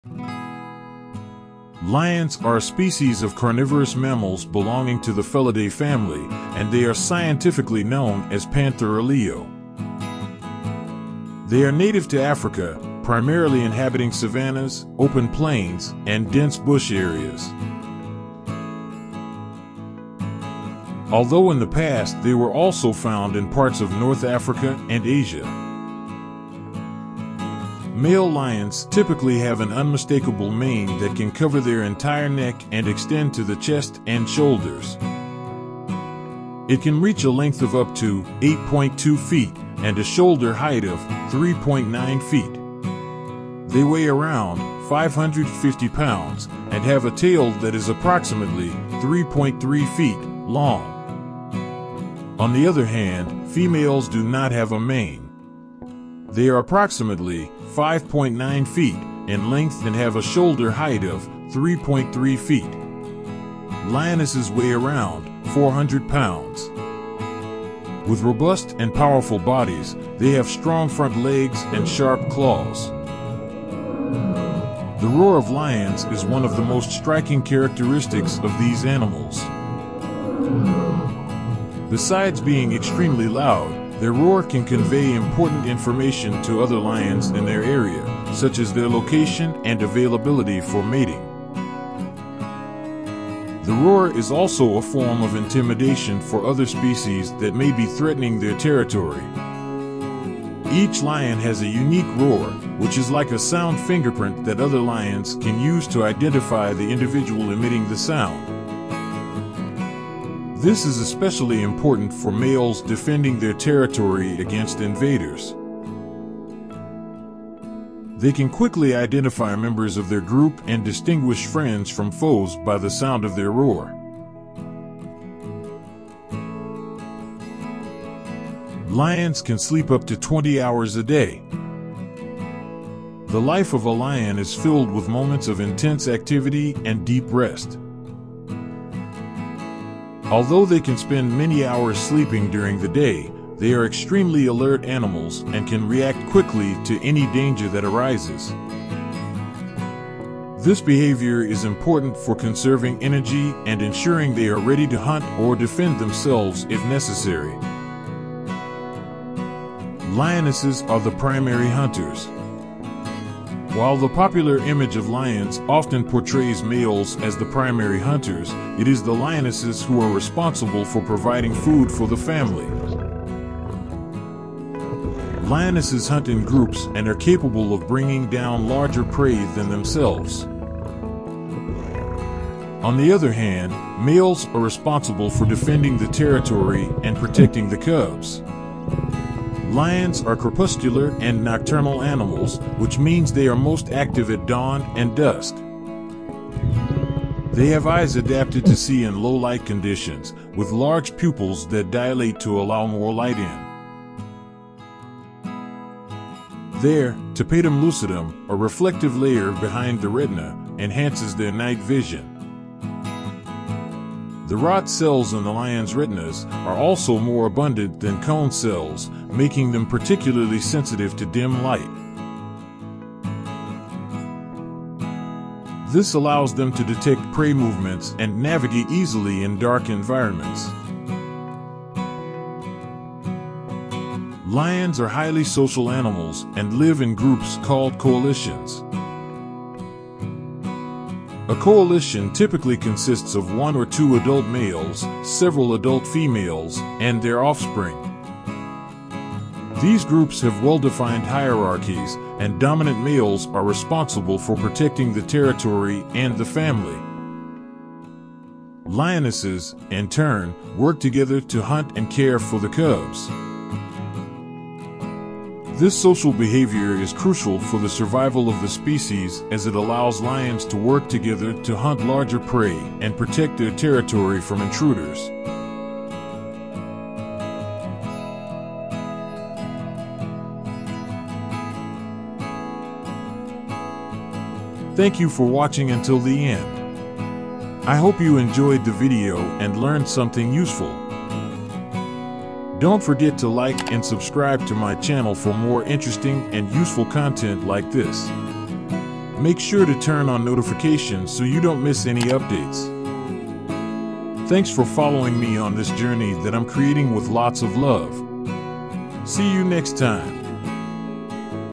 LIONS The King of the Jungle
Lions are a species of carnivorous mammals belonging to the Felidae family, and they are scientifically known as Panthera leo.
They are native to Africa, primarily inhabiting savannahs, open plains, and dense bush areas. Although in the past they were also found in parts of North Africa and Asia.